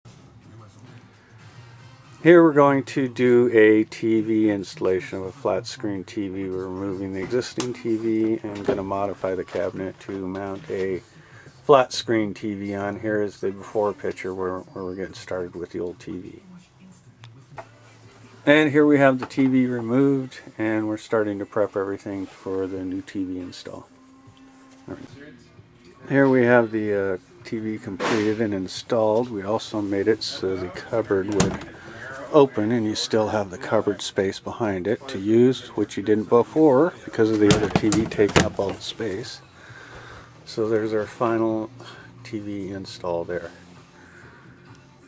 rv flat screen tv installation
remove old tv fill hole and install flat screen tv in motor home jandsrv.com